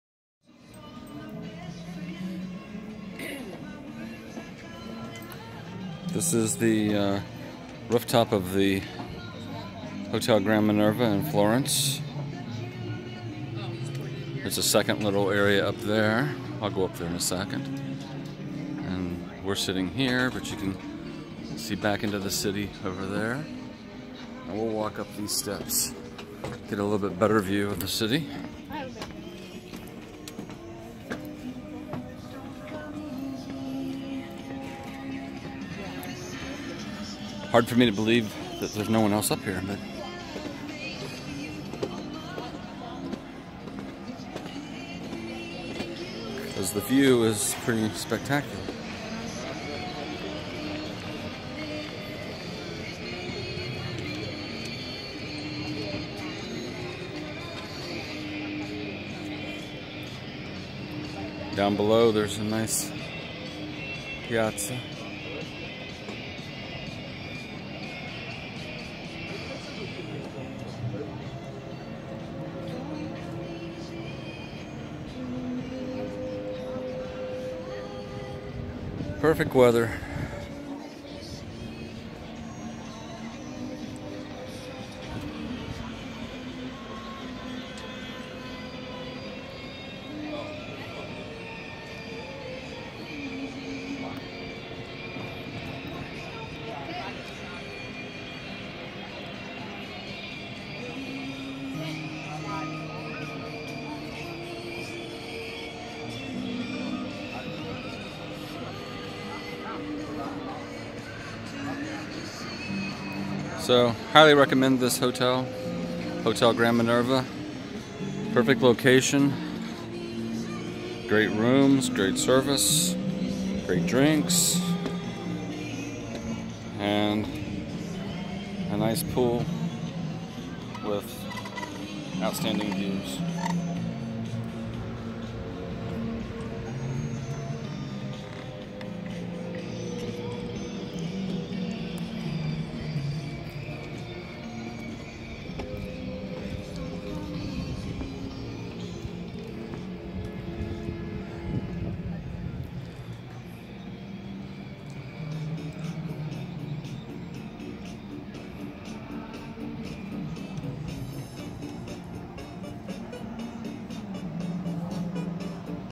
Hotel Grand Minerva - Florence Italy
Terrace view from Hotel Grand Minerva on October 8, 2017.   Nice swimming pool and amazing city views.